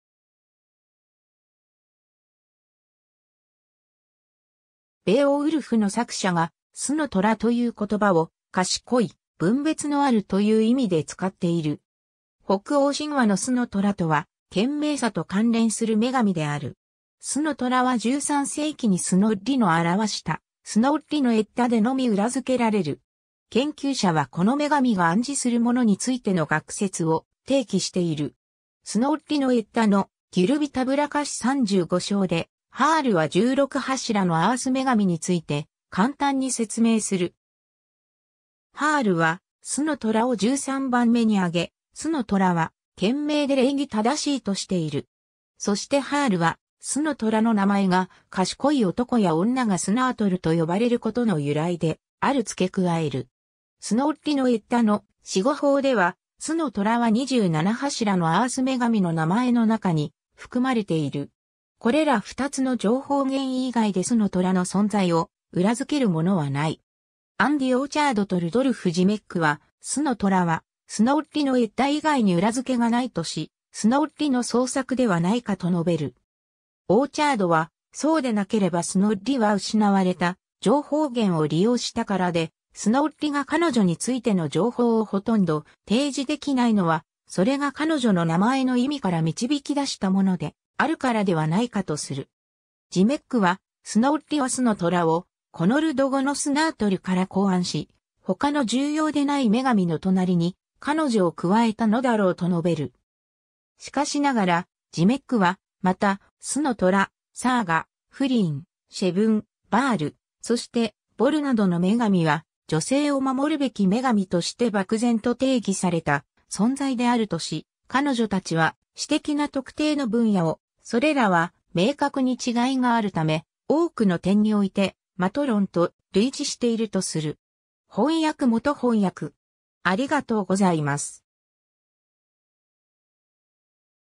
スノトラ
ベーオウルフの作者が「スノトラ（Snotra）」という言葉を「賢い」「分別のある」という意味で使っている。
 北欧神話のスノトラ（古ノルド語: Snotra、「賢い」）とは、賢明さと関連する女神である。
スノトラは13世紀にスノッリの著した『スノッリのエッダ』でのみ裏付けられる。
研究者はこの女神が暗示するものについての学説を提起している。
 『スノッリのエッダ』の「ギュルヴィたぶらかし」35章で、ハールは16柱のアース女神について簡単に説明する。
ハールはスノトラを13番目に挙げ、スノトラは「賢明で礼儀正しい」としている。
そしてハールはスノトラの名前が、賢い男や女が snotr と呼ばれることの由来である付け加える。
『スノッリのエッダ』の「詩語法」では、スノトラは27柱のアース女神の名前の中に含まれている。
これら2つの情報源以外でスノトラの存在を裏付けるものはない。
 アンディ・オーチャードとルドルフ・ジメックは、スノトラは『スノッリのエッダ』以外に裏付けがないとし、スノッリの創作ではないかと述べる。
オーチャードは、そうでなければスノッリは失われた情報源を利用したからで、スノッリが彼女についての情報をほとんど提示できないのは、それが彼女の名前の意味から導き出したものであるからではないかとする。
 ジメックはスノッリはスノトラを古ノルド語の snotr （賢い）から考案し、「他の重要でない女神の隣に彼女を加えた」のだろうと述べる 。
しかしながら、ジメックはまた、スノトラ、サーガ、フリーン、シェヴン、ヴァール、そしてヴォルなどの女神は「女性を守るべき女神」として漠然と定義された存在であるとし、彼女たちは「私的な特定の分野を（守るが）、それらは明確に違いがあるため、多くの点においてマトロンと類似している」とする。
 翻訳元 翻訳